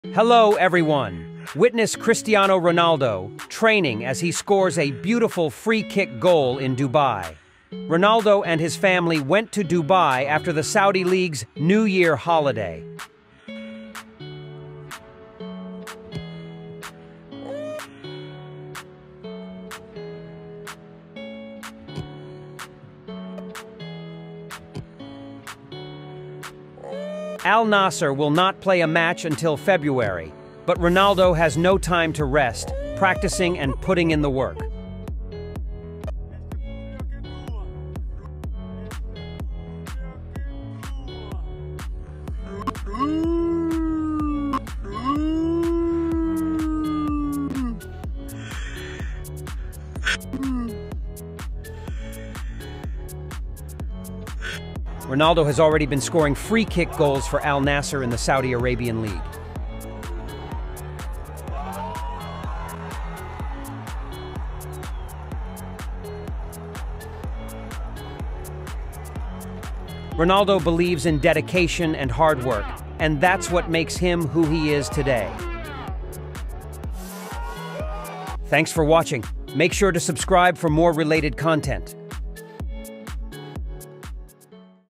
Cristiano Ronaldo Free Kick Goal in Dubai Training!
Witness Cristiano Ronaldo training as he score a beautiful free kick goal in Dubai. Ronaldo and his family are went to dubai after Saudi league new year holiday. Cristiano Ronaldo Free kick Goal 2024 and Cristiano Ronaldo First goal 2024.